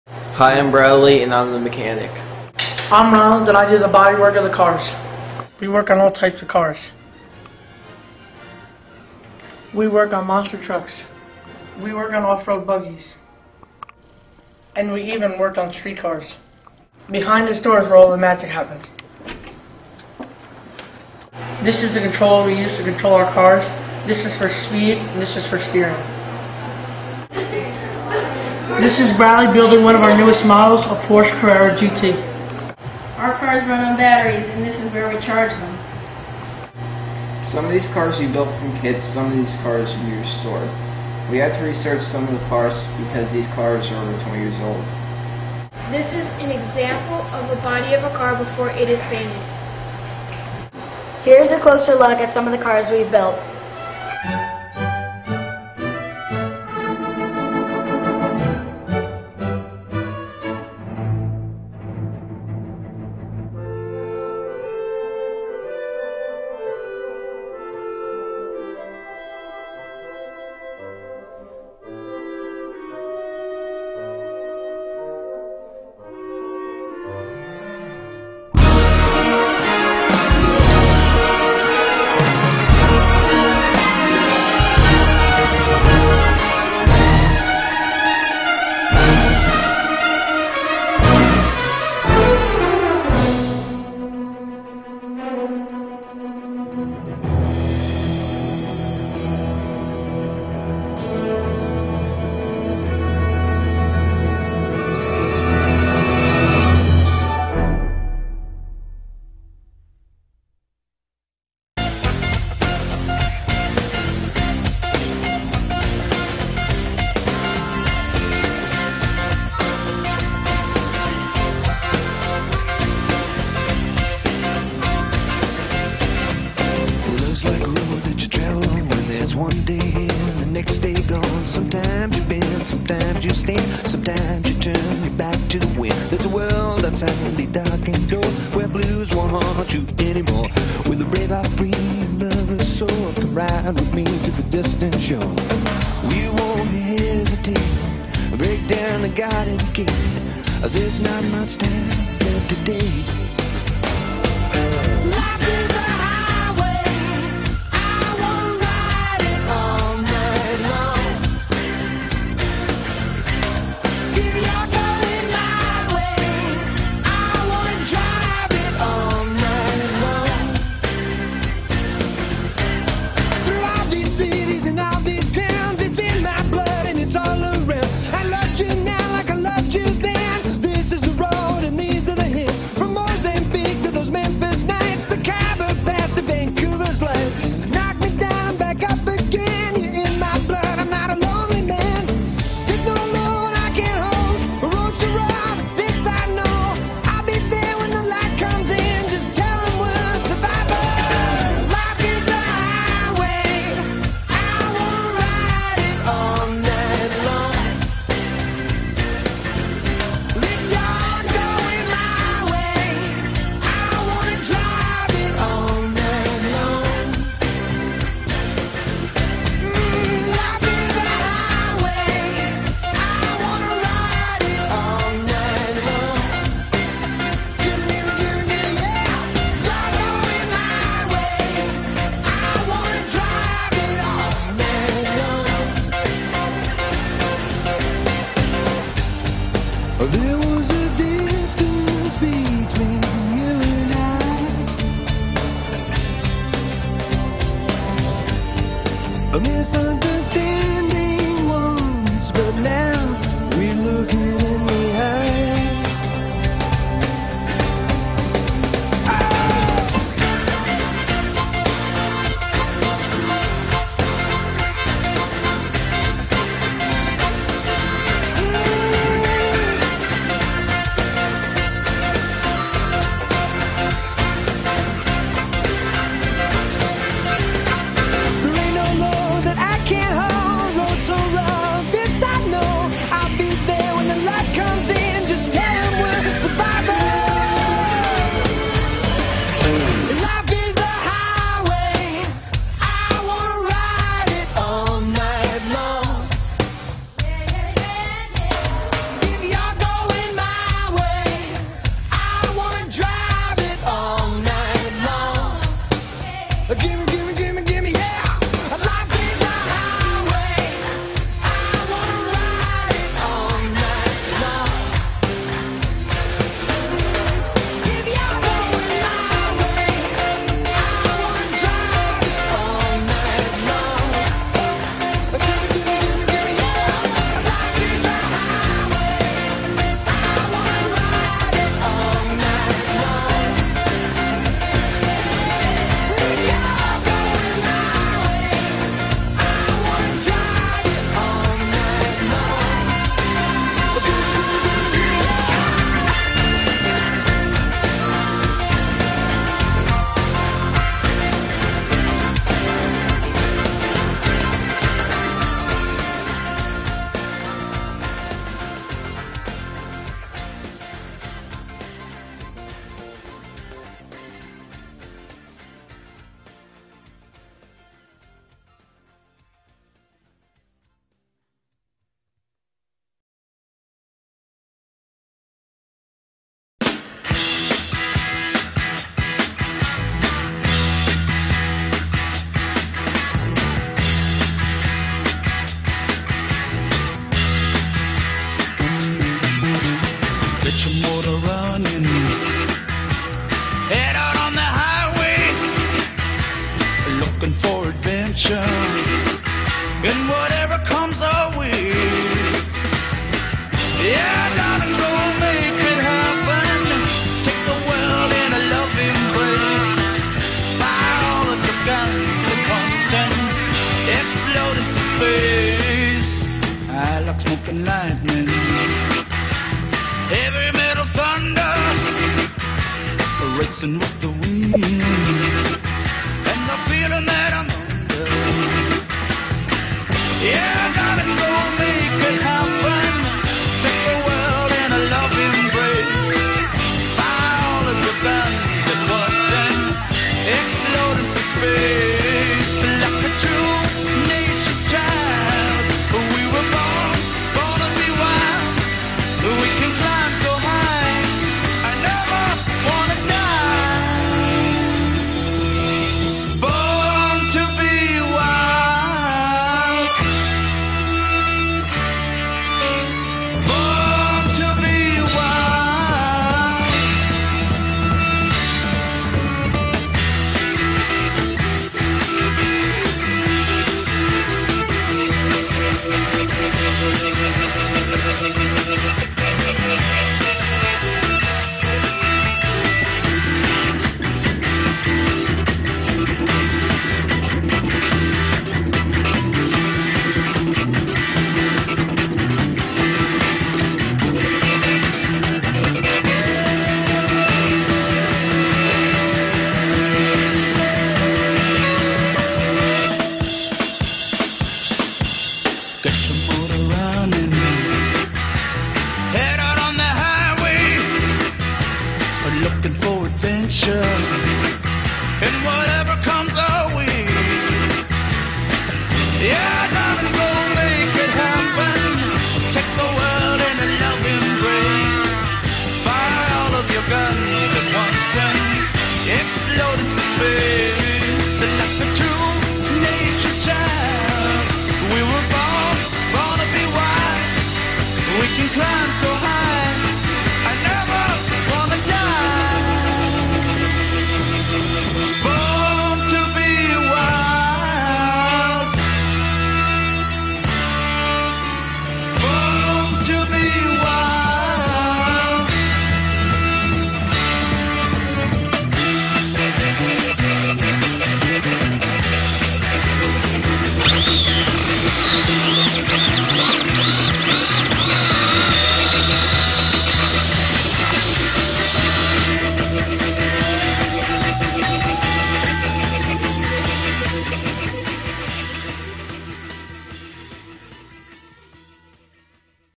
Our Cars
Take a look at these awesome radio control car kits.  Some of these are new, while others are restorations of vintage models.